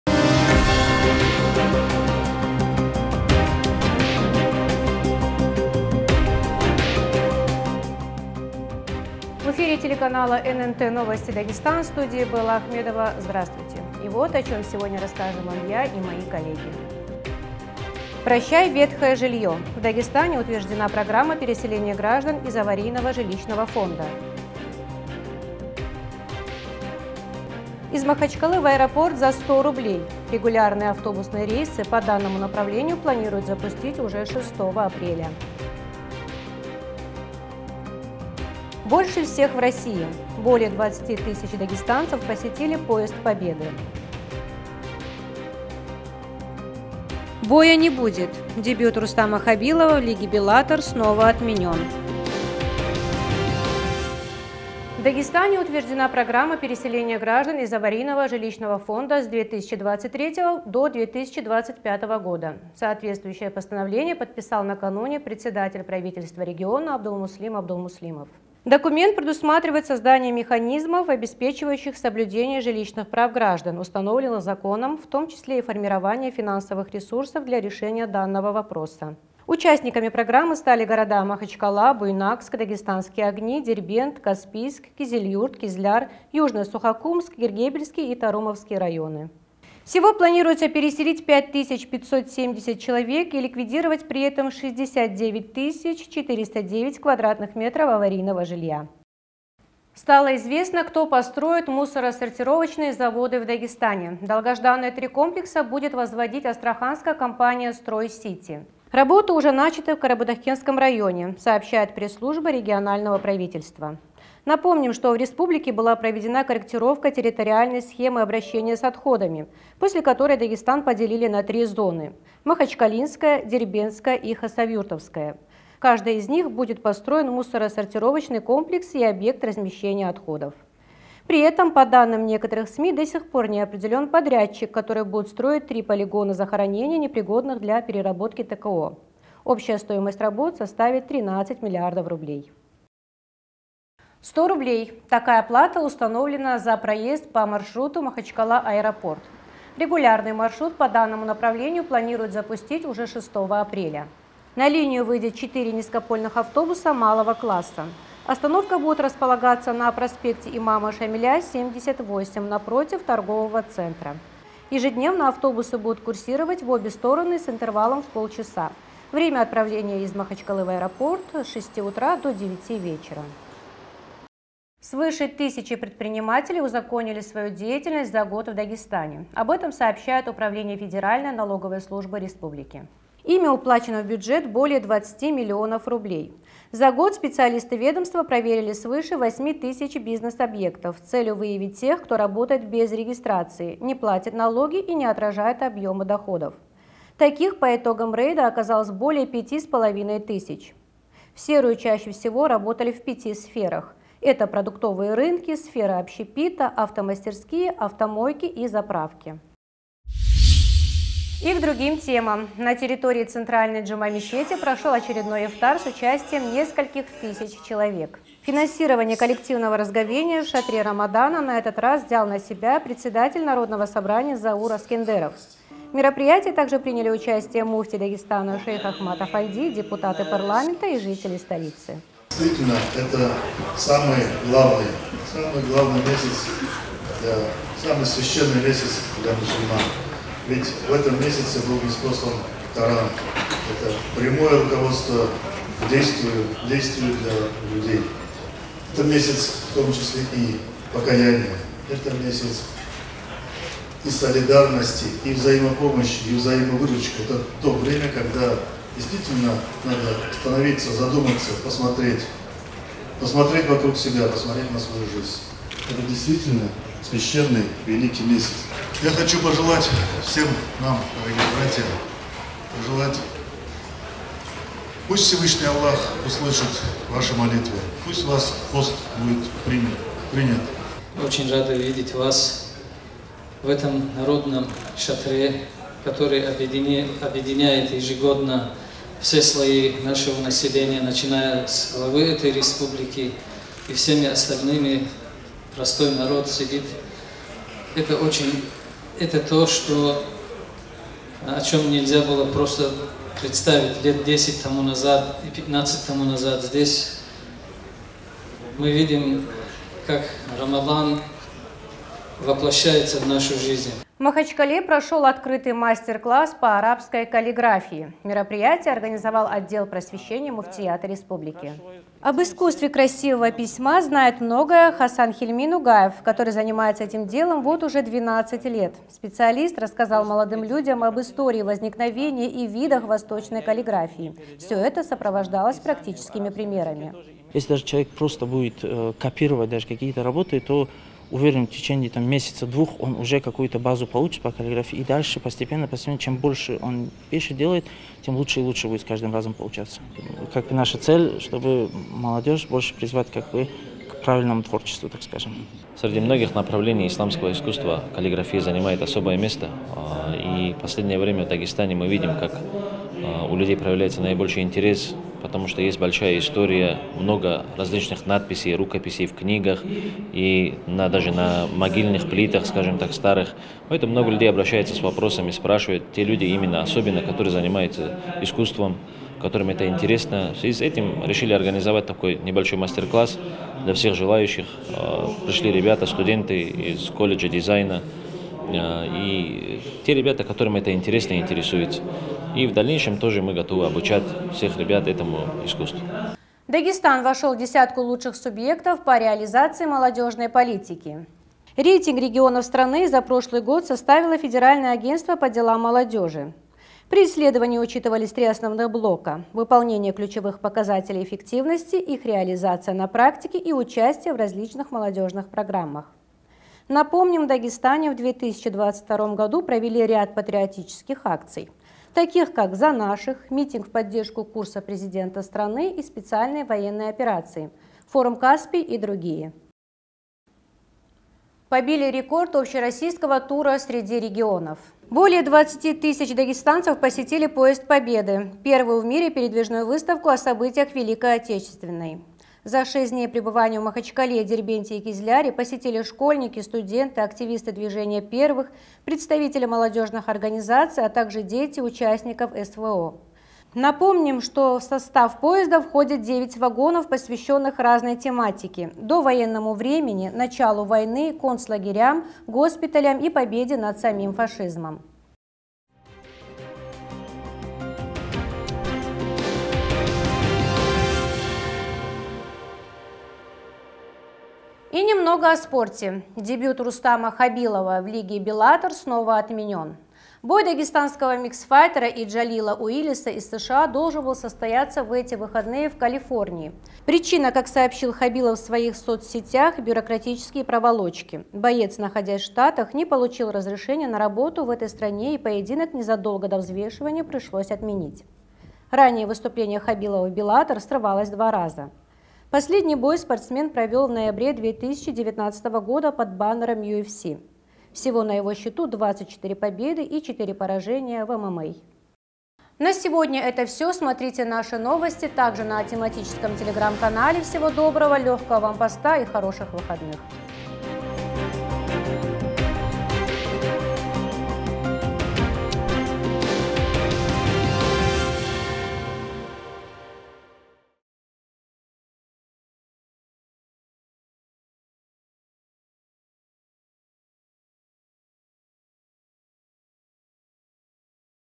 Новости Дагестана за 31.03.2023
1.Прощай ветхое жилье.  В Дагестане утверждена программа переселения граждан из аварийного жилищного фонда .

2. Из Махачкалы в Аэропорт за 100 рублей. Регулярные автобусные  рейсы по данному направлению планируют запустить уже 6 апреля.

3. Больше всех в России ! Более 20 тысяч дагестанцев посетили «Поезд Победы»

4. Боя не будет!  Дебют Рустама Хабилова в лиге Bellator снова отменен.